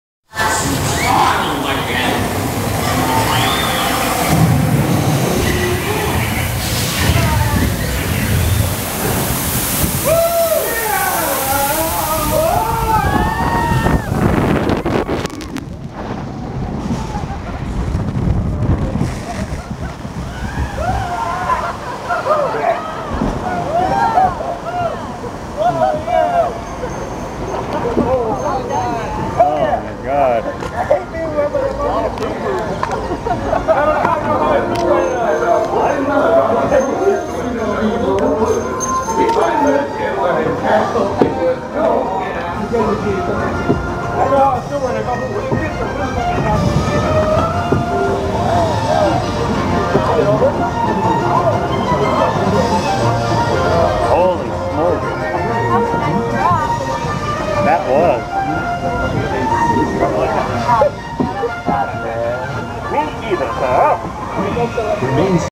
Dudley Do Right's Ripsaw Falls Universal Studios, Orlando
In this video we're just finishing up the last part of the Dudley Do Right's Ripsaw Falls ride at Universal Studios in Orlando, Florida.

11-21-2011